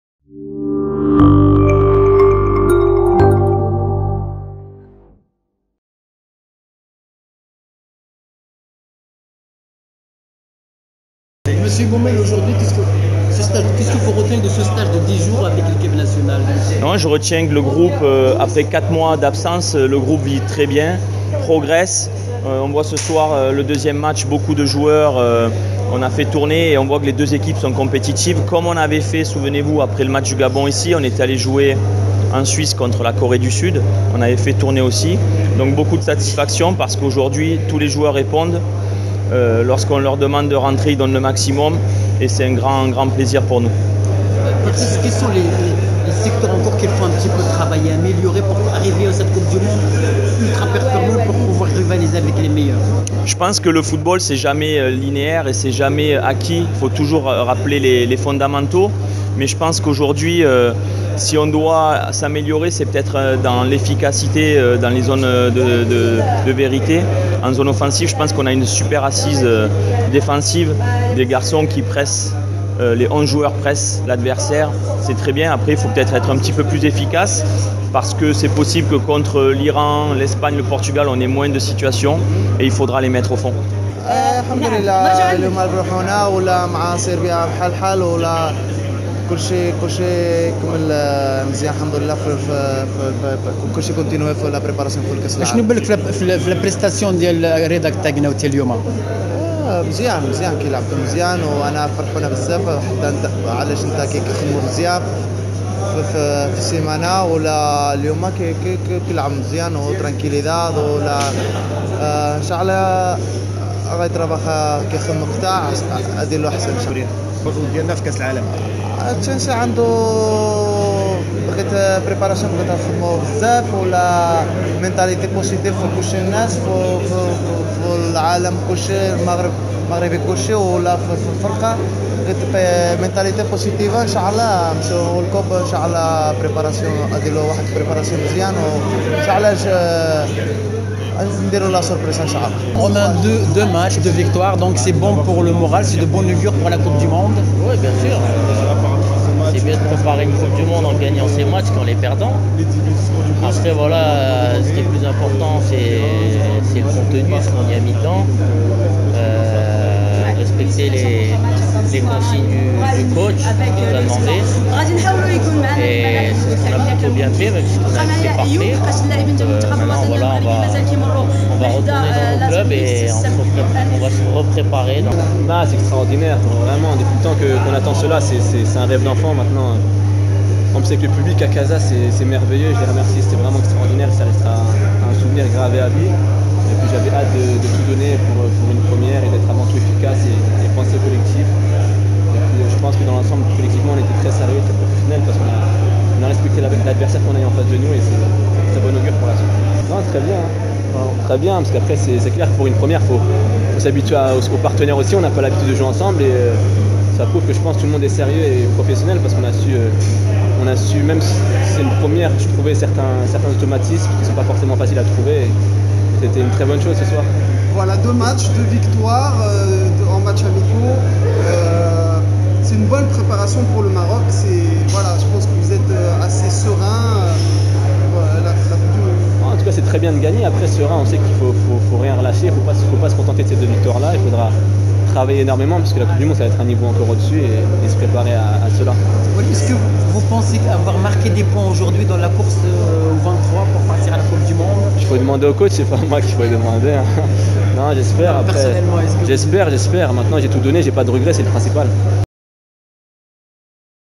Maroc - Ouzbékistan : Déclarations d'après match
Patrice Beaumelle, Romain Saïss, Oualid El Hajjam et Munir El Muhamadi ont affiché leur satisfaction après la victoire face à l’Ouzbékistan. Les joueurs assurent que ce succès est bon pour le moral avant de débuter la préparation pour la Coupe du monde.